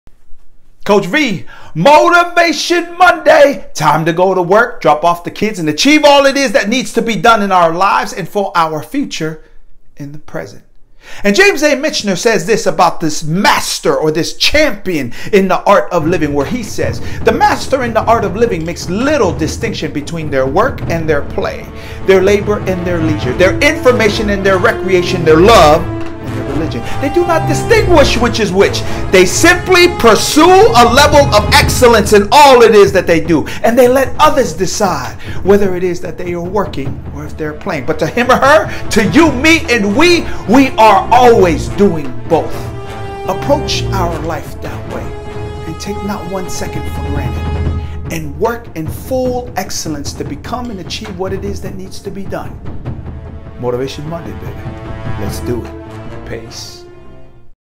Motivation Monday by Coach V
Wishing you a great motivation Monday for this Thanksgiving week. Be the master in your art of living. Be motivated, put in the action and work, and live your best life.

Coach V